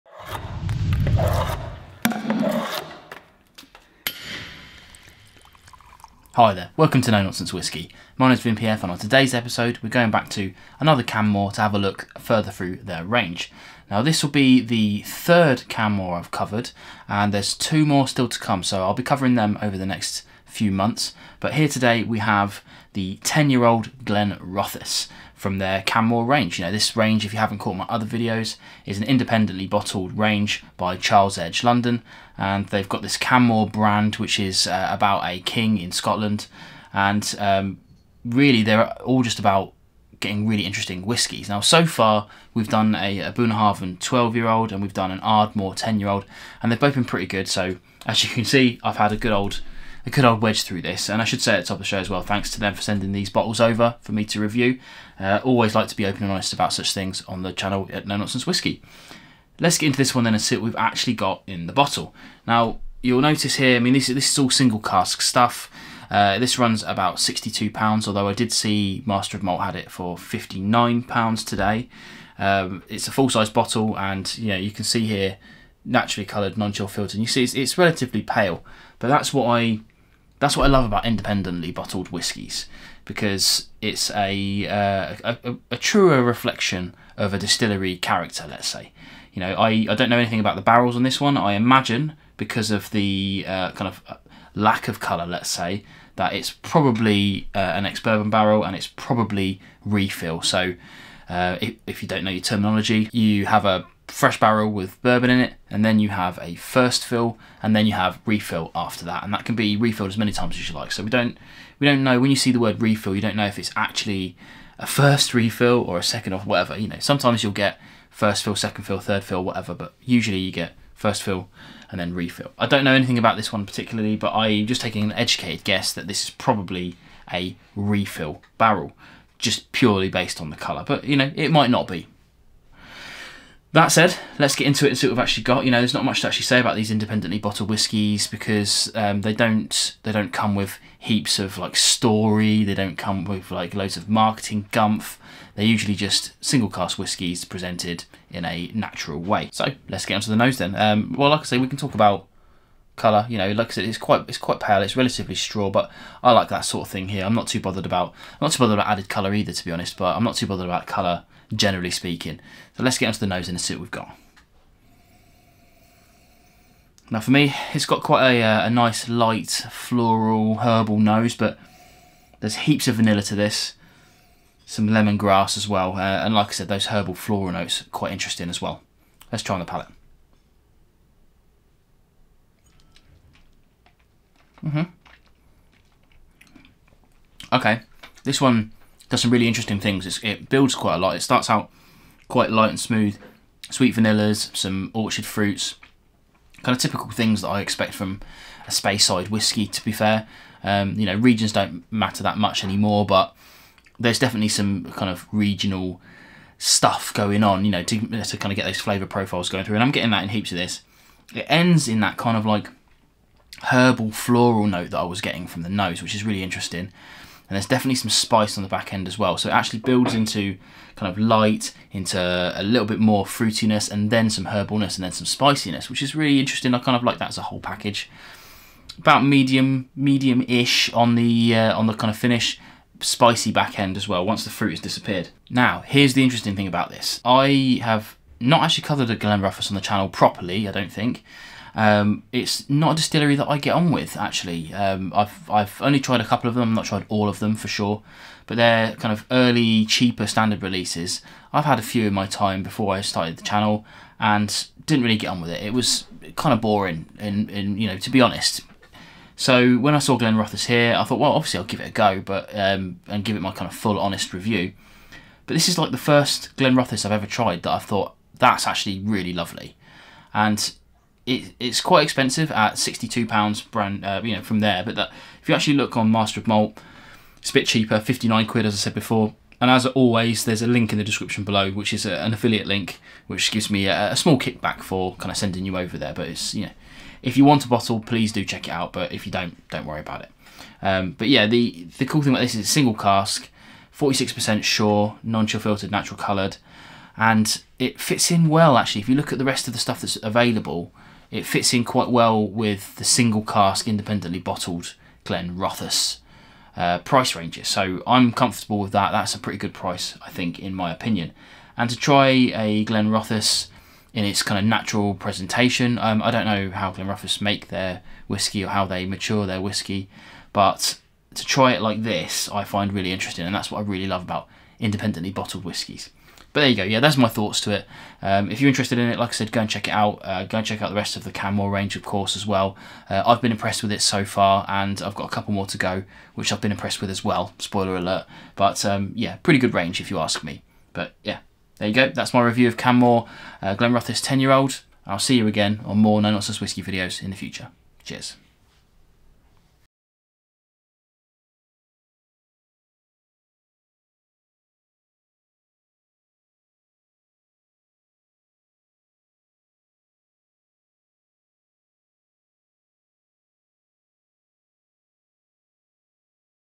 Canmore Glenrothes 10yr | BETTER than the CORE
Thanks for checking out my review of the Canmore Glenrothes 10.

Chapters:
0:00 Intro

All whiskies are reviewed completely honestly regardless of origin.

Please enjoy your alcohol responsibly. Must be of legal drinking age in your country.

Ring Light
Main Room Light